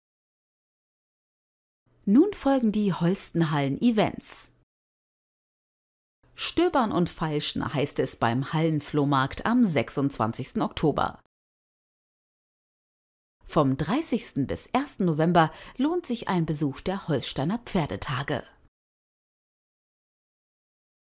Holsteiner Pferdetag | Events im Überblick #41 | Holstenhallen Neumünster
Holsteiner Pferdetag - Events im Überblick

Termine in dieser Ausgabe:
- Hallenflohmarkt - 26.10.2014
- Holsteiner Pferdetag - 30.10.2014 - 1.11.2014